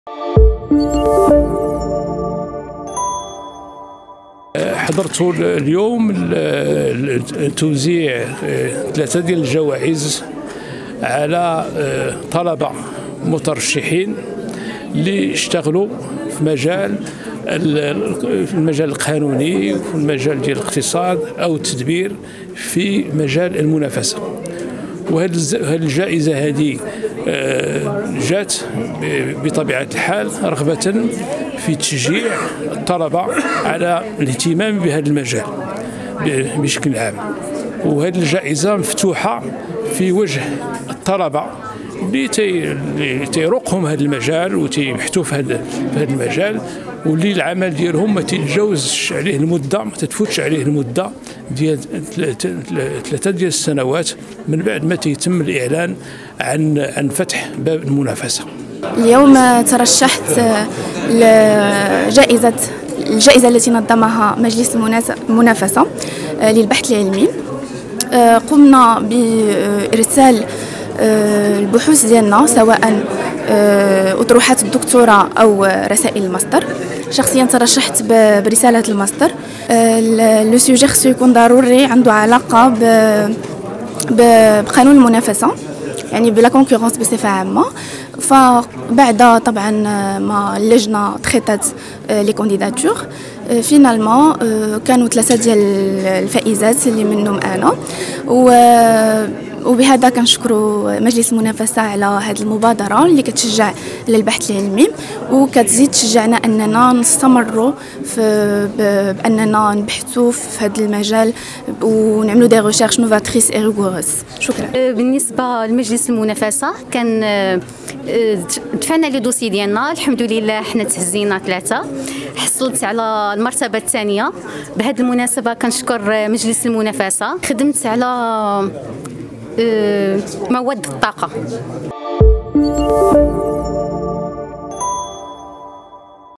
مجلس المنافسة ينظم لقاءه السنوي ويقدم جائزة " البحث"
نظم مجلس المنافسة اليوم الخميس 15 فبراير 2024 لقاءه السنوي، حيث تم تقديم جائزة "مجلس المنافسة للبحث"